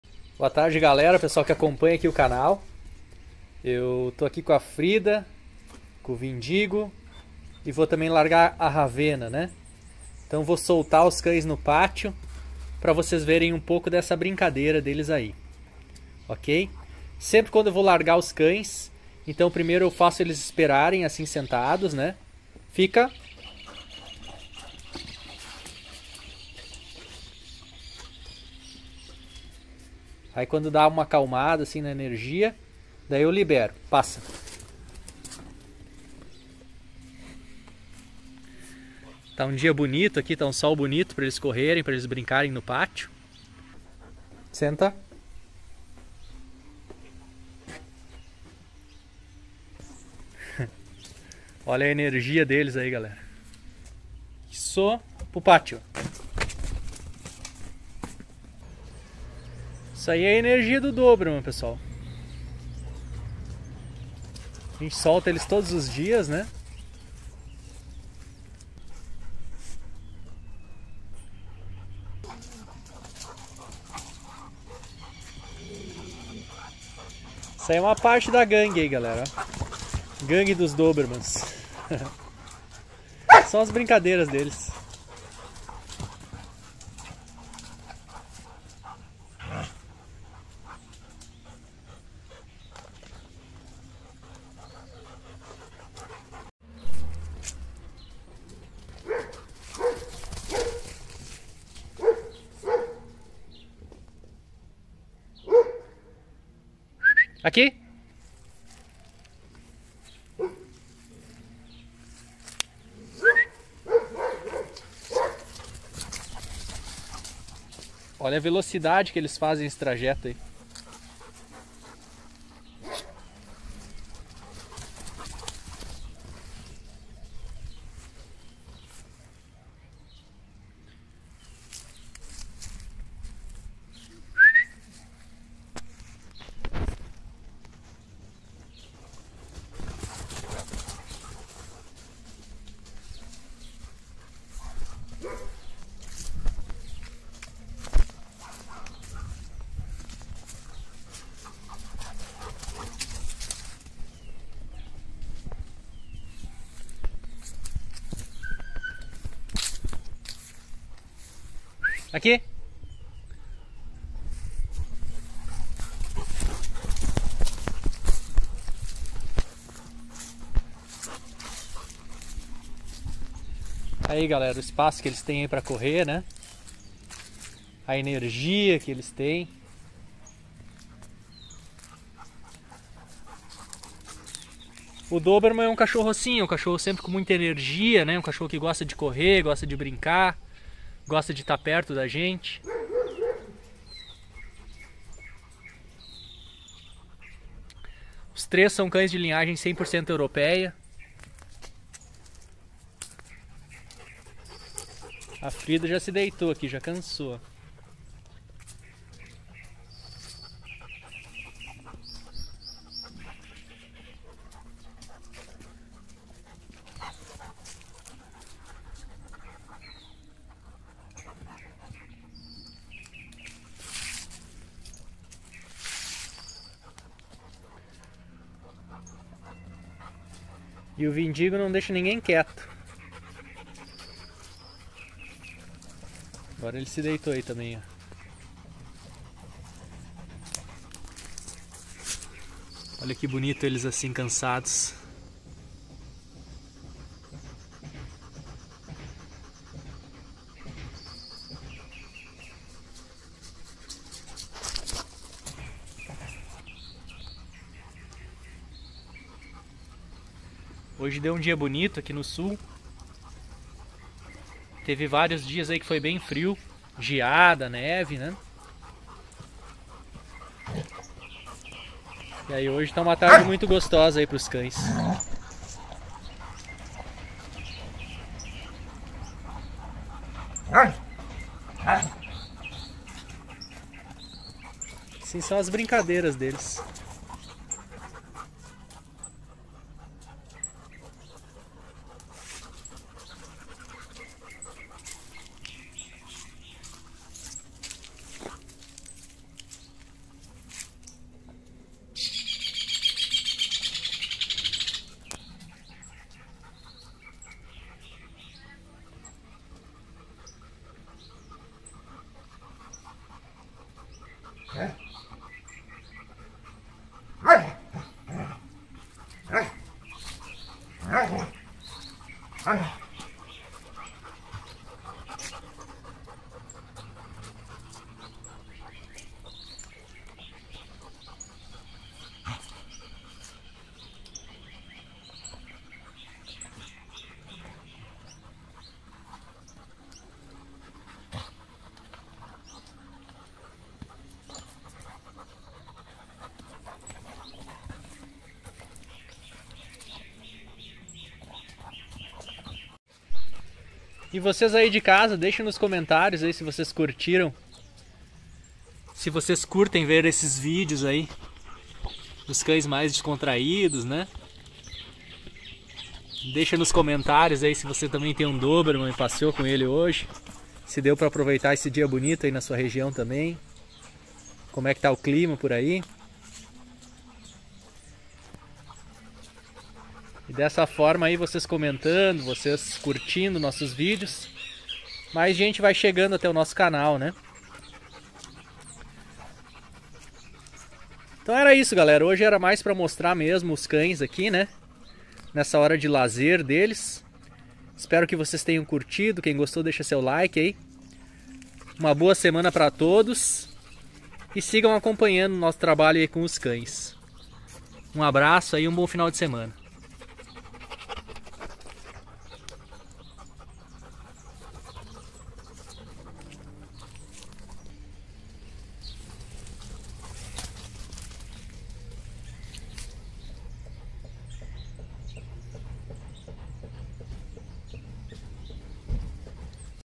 Dobermann's apostando corrida!
Momento de descontração dos cães correndo no pátio e aproveitando um belo dia de sol!